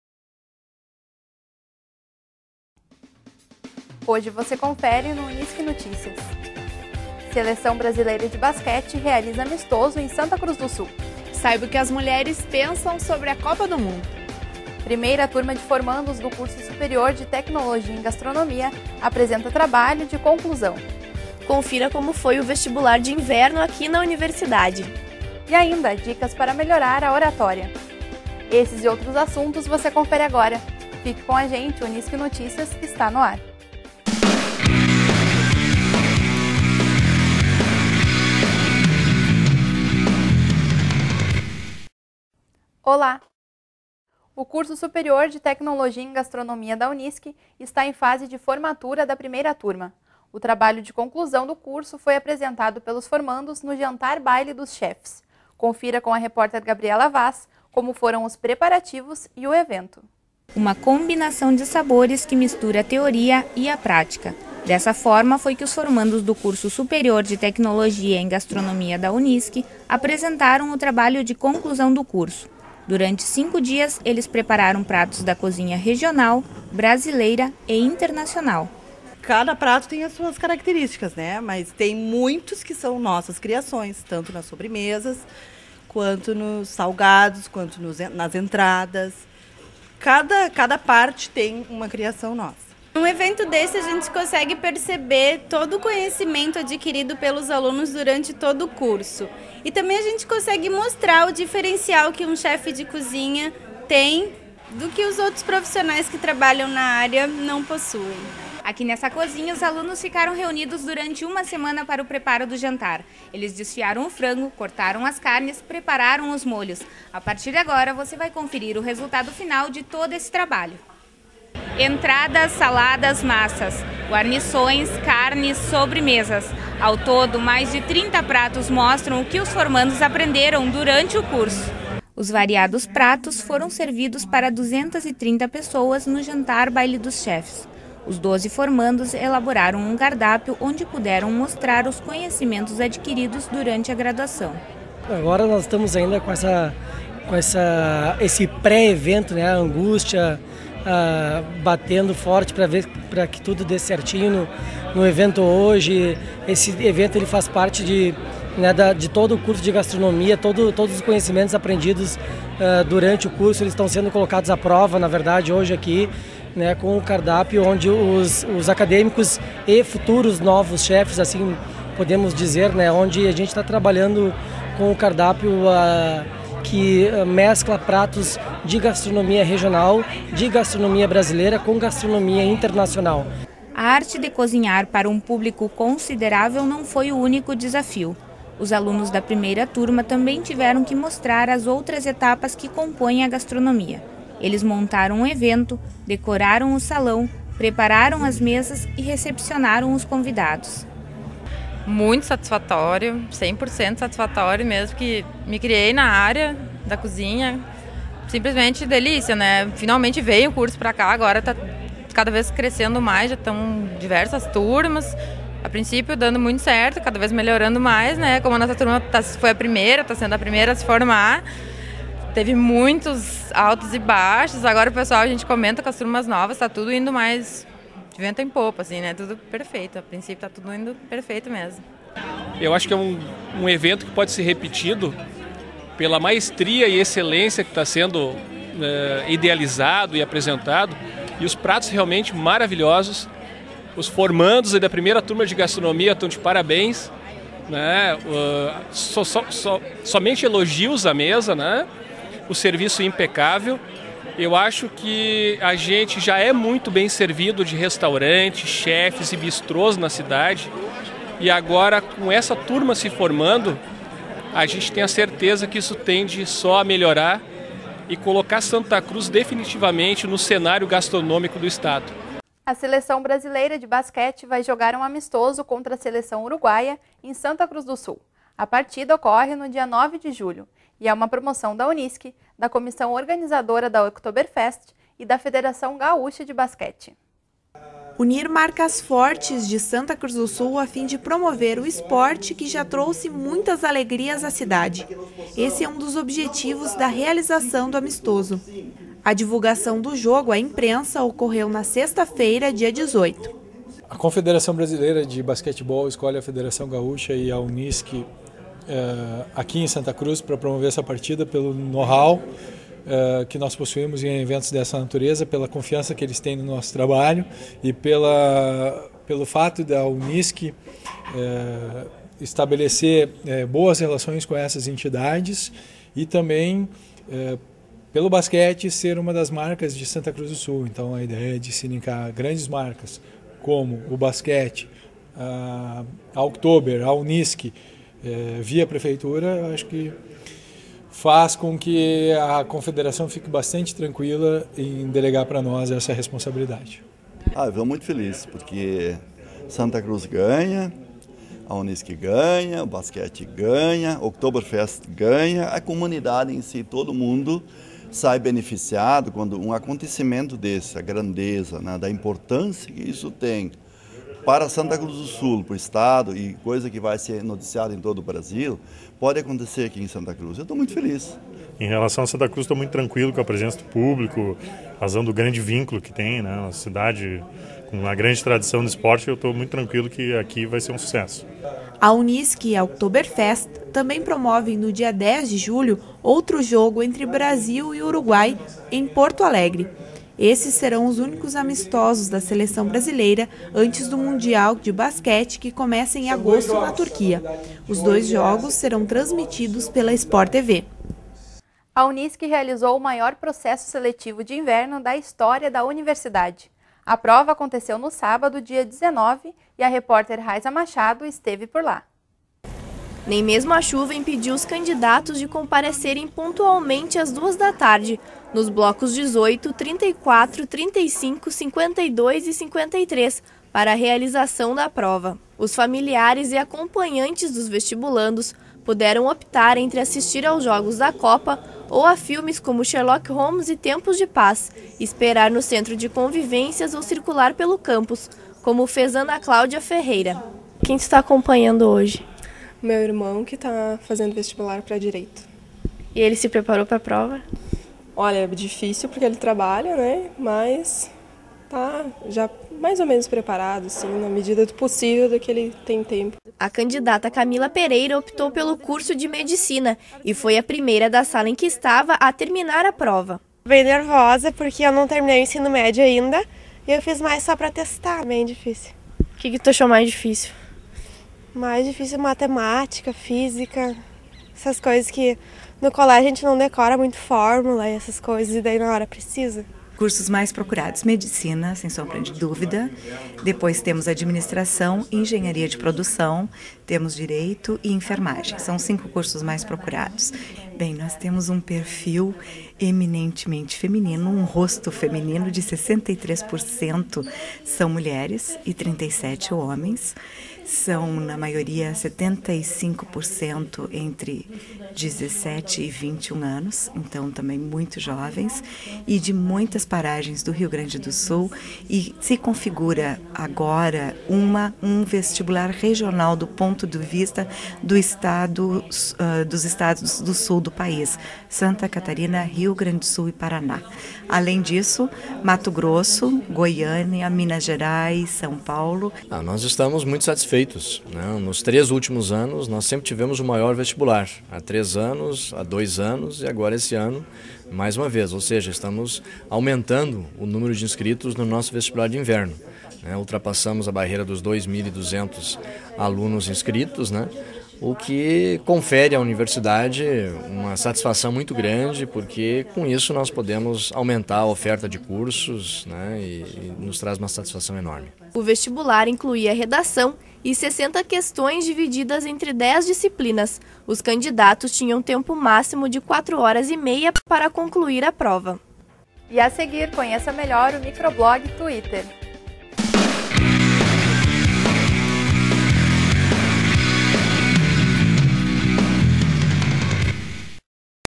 Unisc Notícias - 25/06/10 - Bloco 1
Bloco 1
Primeira turma do curso superior de tecnologia em gastronomia
apresenta trabalho de conclusão.
Seleção brasileira de basquete realiza amistoso em Santa Cruz do Sul.
Vestibular de inverno é o maior da história da Unisc.